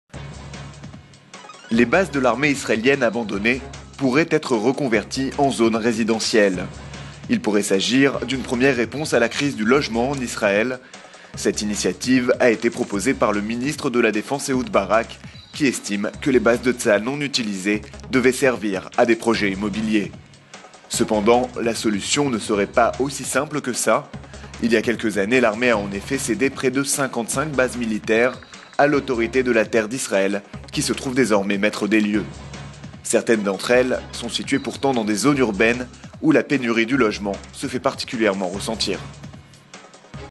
Les vieilles bases de Tsahal pour résoudre la crise du logement ?
Les bases de l'armée israélienne abandonnées pourraient être reconverties en zones résidentielles. Il pourrait s'agir d'une première réponse à la crise du logement en Israël. Cette initiative a été proposée par le ministre de la défense Ehoud Barak, qui estime que les bases de Tsahal non utilisées devaient servir à des projets immobiliers. Cependant, la solution ne serait pas aussi simple que ça. Il y a quelques années, l'armée a en effet cédé près de 55 bases militaires à l'Autorité de la terre d'Israël qui se trouve désormais maitre des lieux. Certaines d'entre elles sont situées dans des zones urbaines où la pénurie du logement se fait particulièrement ressentir.